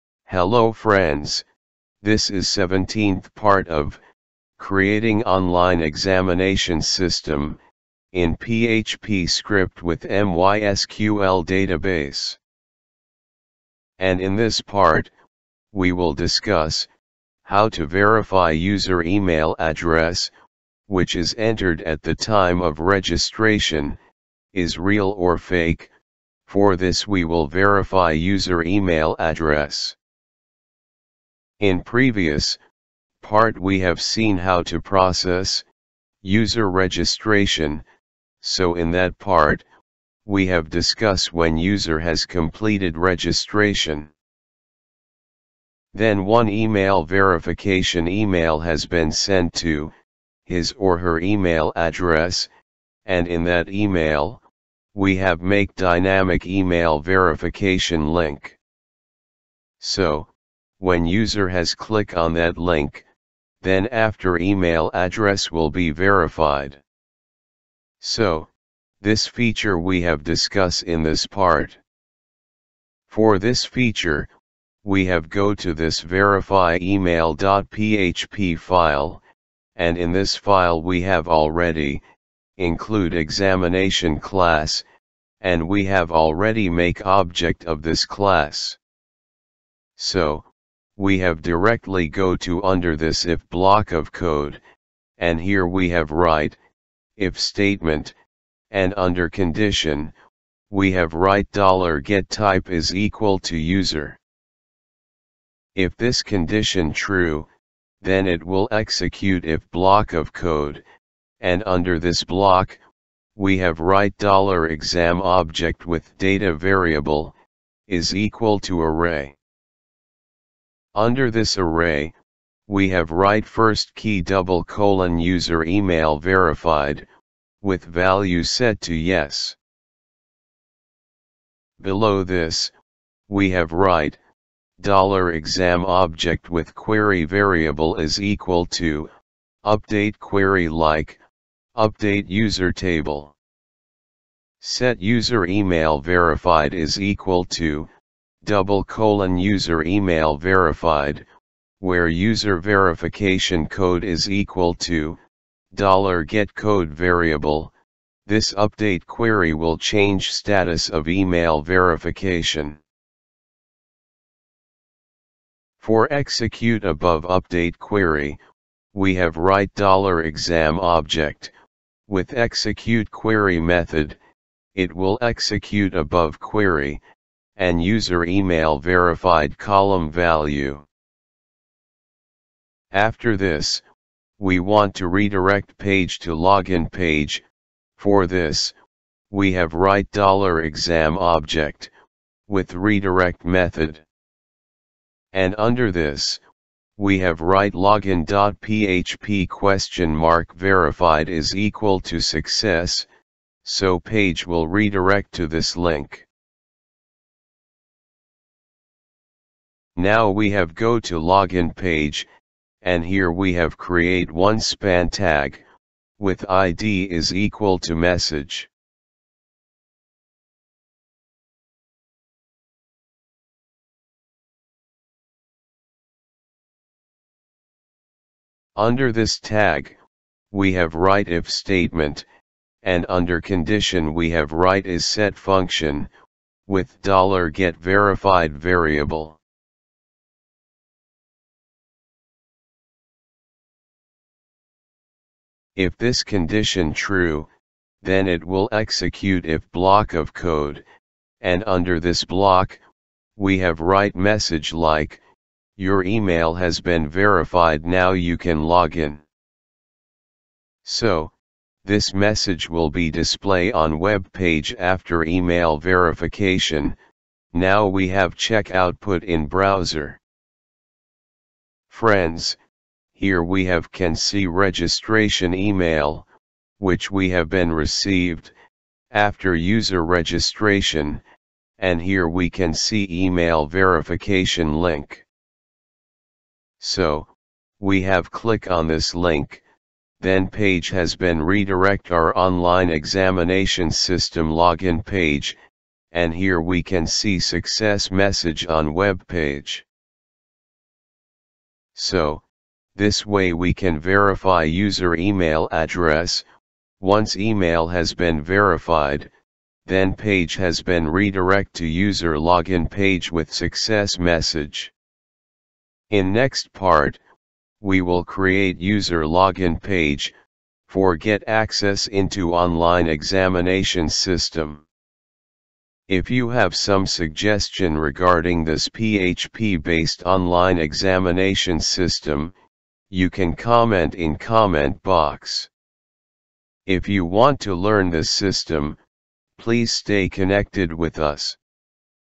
17 - User Email Address Verification - Online Examination System in PHP
This verified user can take part in Online exam of this system.